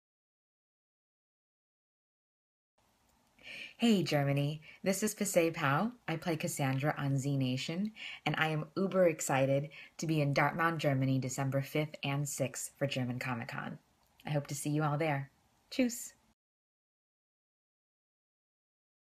Videomessage Pisay Pao - German Comic Con 2015
Freut euch am 5. & 6. Dezember auf Pisay Pao aka Cassandra aus ZNation auf der GERMAN COMIC CON in den Westfalenhallen Dortmund! Hier ein kleiner Gruß vorab ;)